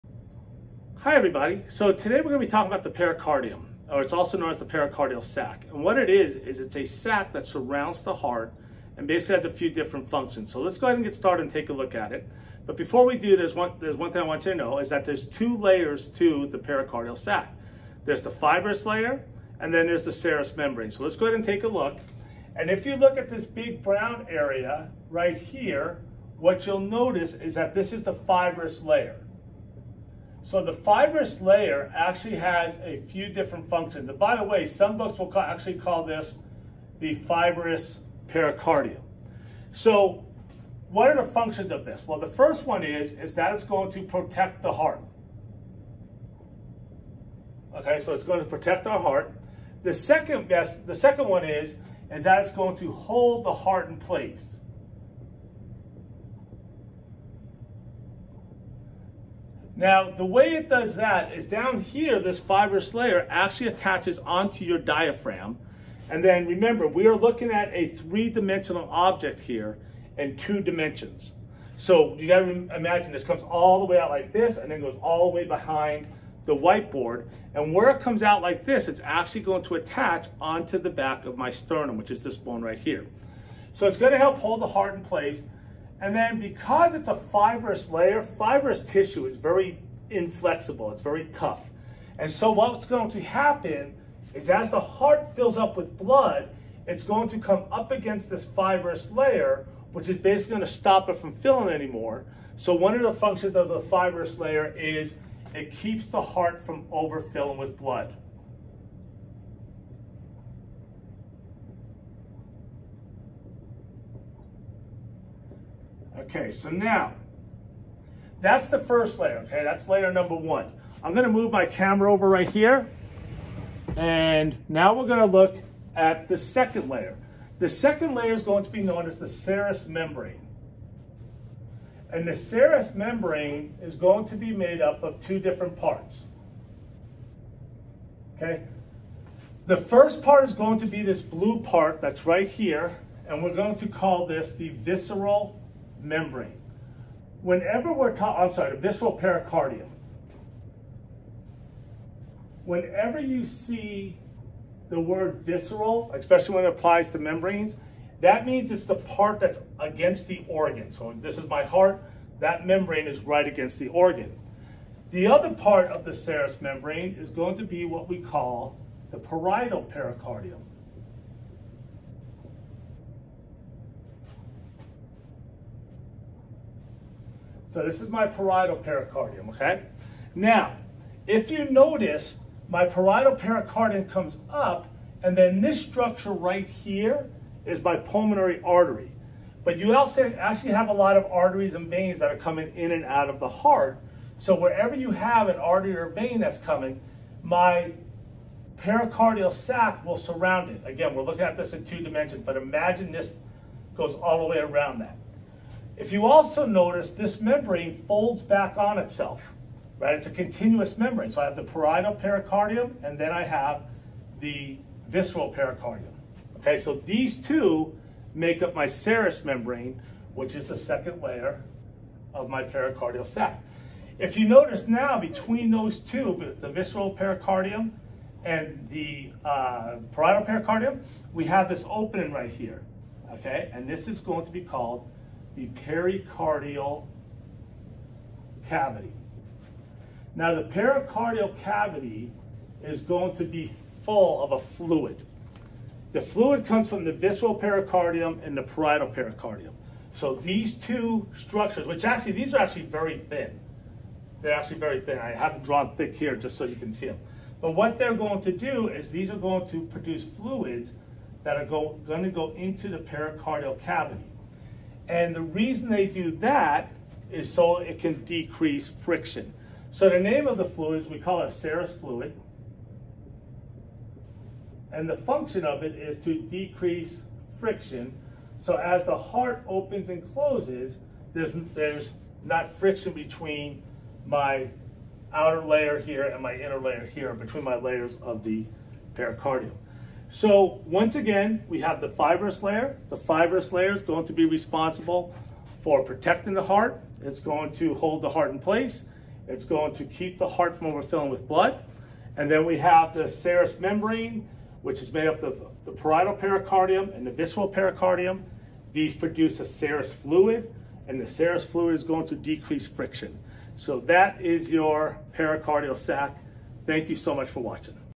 Pericardium Anatomy and Physiology
Describes the pericardium, including the two layers and their functions.